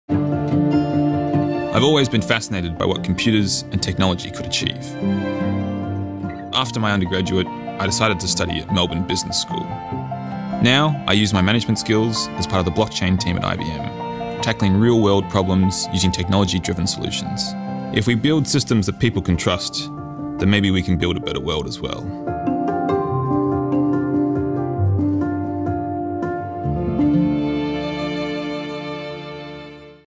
Make your next step a leap - Melbourne Business School
Billy O'Connor took his learnings from Melbourne Business School (MBS) to IBM, where he joined the blockchain team, exploring uncharted territory and developing new, more trustworthy systems.

Make your next step a leap.